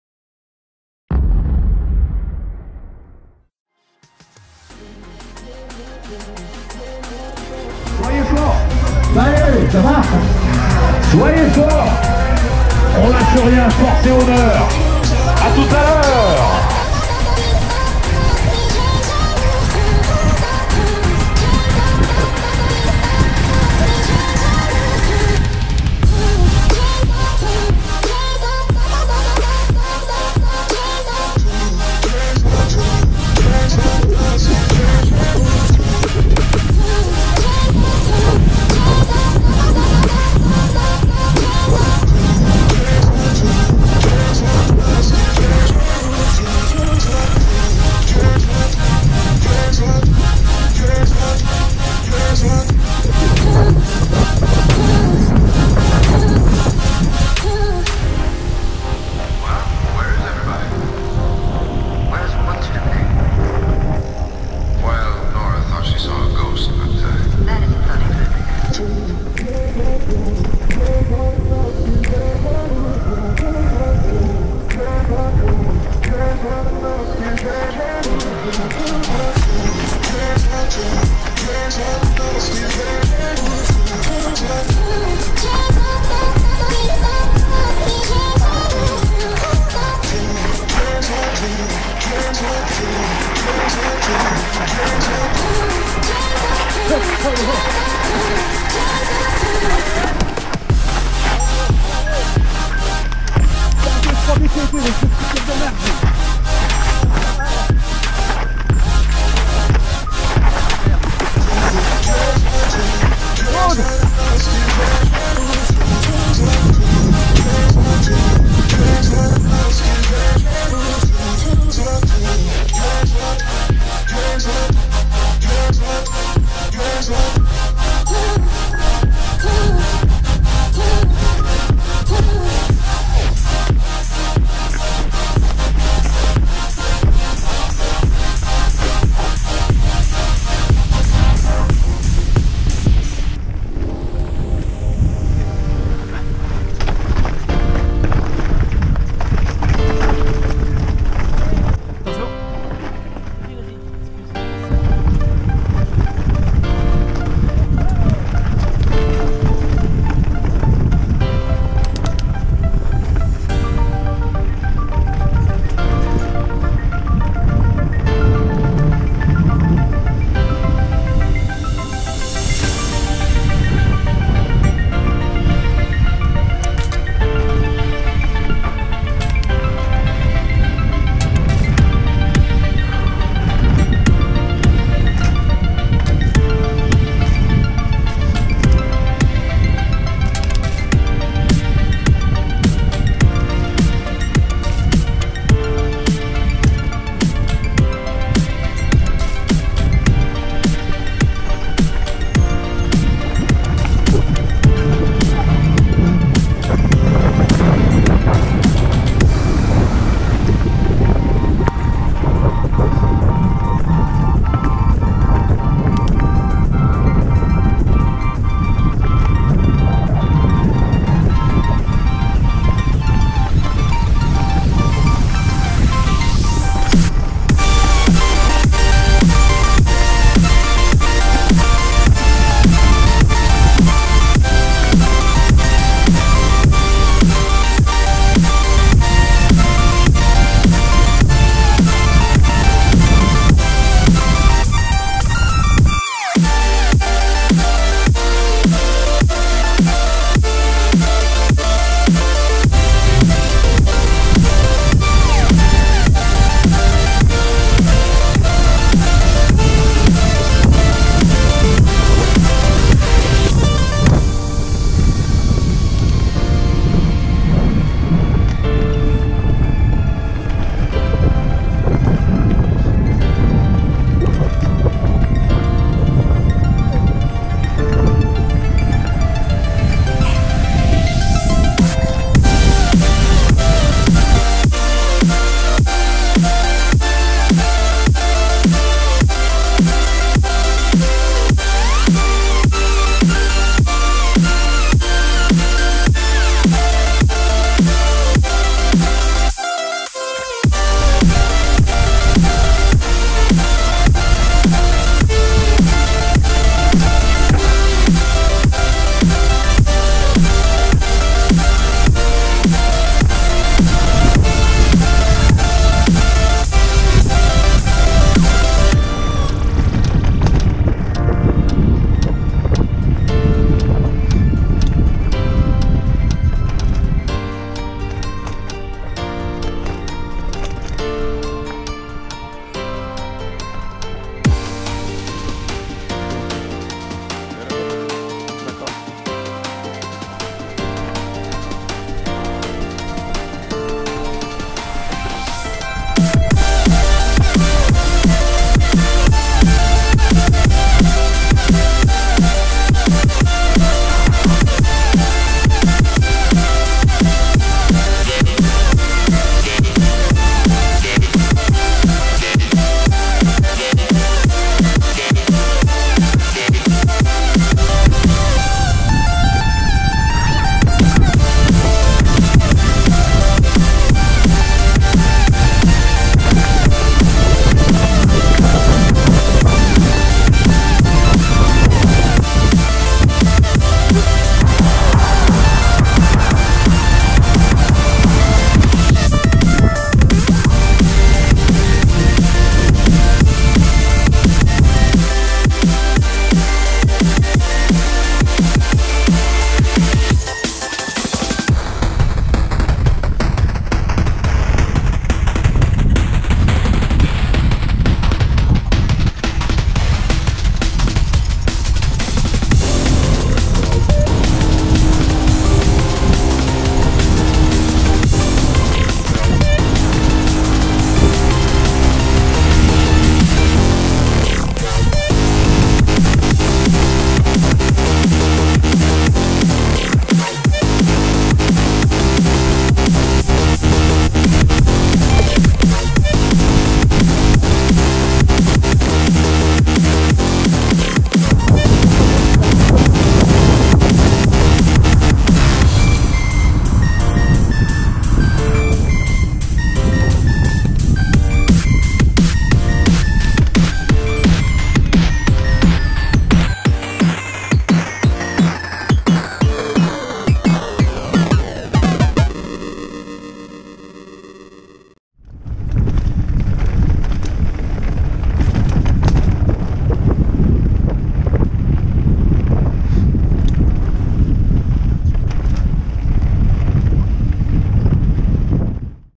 VTT - Isostar Master Roc (Roc d'Azur 2017)
Vidéo of my participation to the Isostar Master Roc of the Roc d'Azur 2017 in Fréjus, South of France.

Vidéo: Filmed with Go Pro Hero Session 5